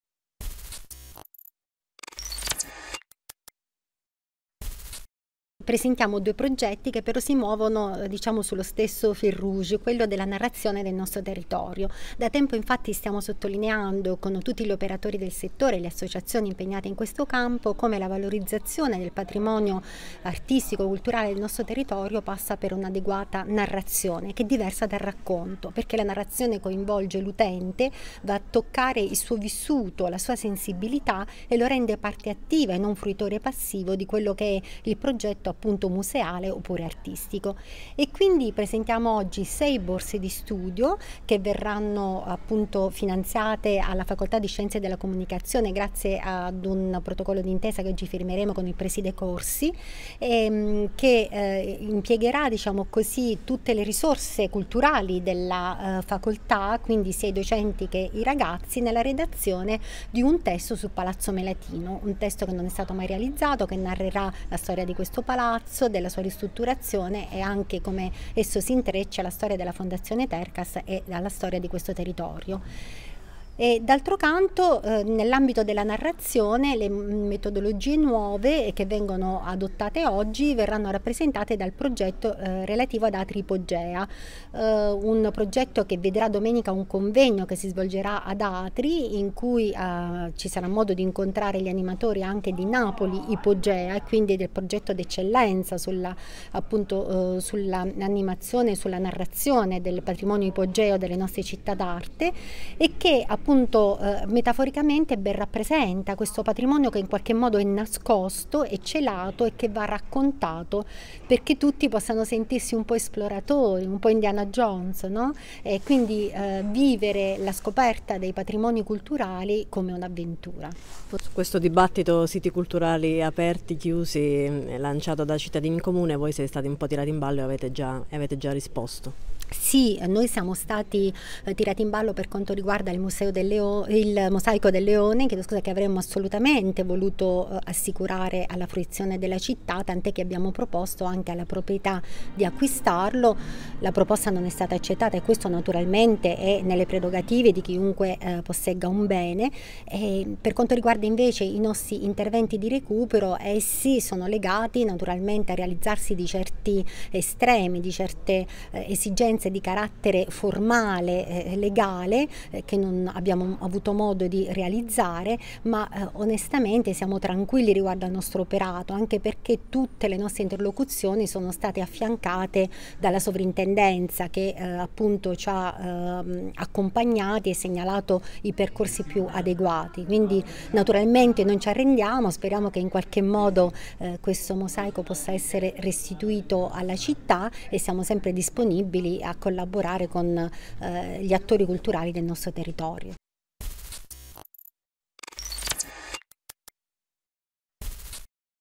Progetti per la valorizzazione di preziosi beni culturali tra Palazzo Melatino e Atri sotterranea
Due progetti rivolti alla valorizzazione di straordinari beni culturali del nostro territorio: Palazzo Melatino a Teramo, sede della Fondazione Tercas ed Atri Sotterranea